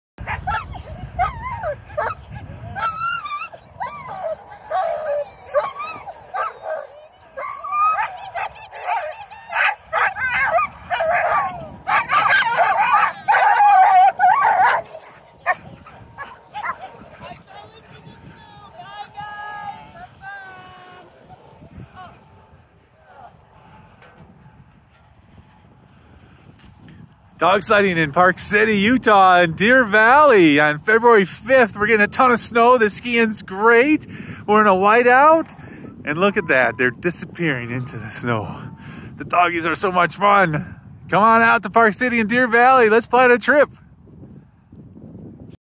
Utah Dog Sledding in February 2023
Enhance your Utah Winter Experience with a Dog Sledding Tour in the Wilderness!  The dogs are so much fun!!  They want to run and run and run.   If they aren't running, they are happy to take pictures!